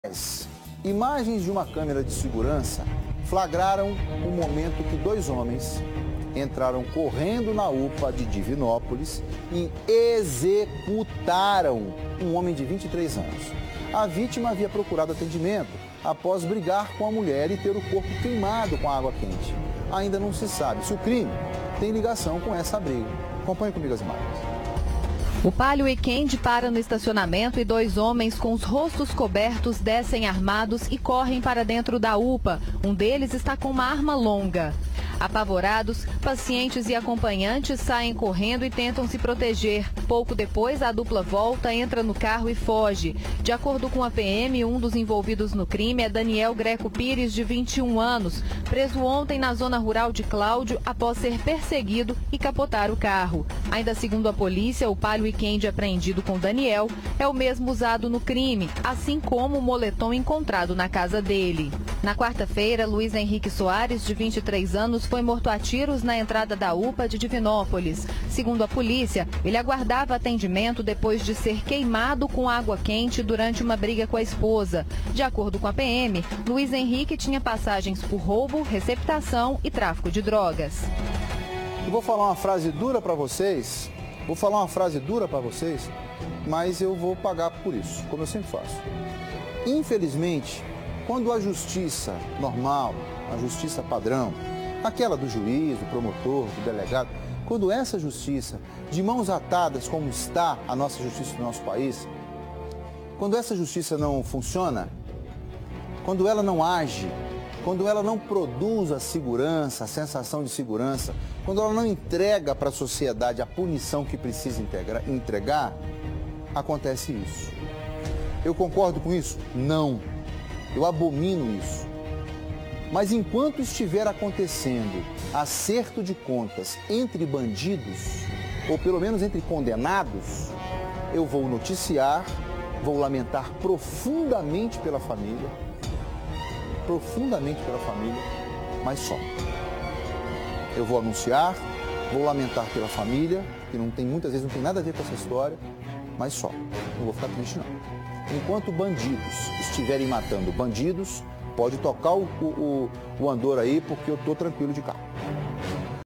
Homens matam com vários tiros um jovem dentro da UPA
Câmeras de segurança flagraram o momento em que dois homens entram correndo dentro de UPA em Divinópolis e executam jovem de 23 anos.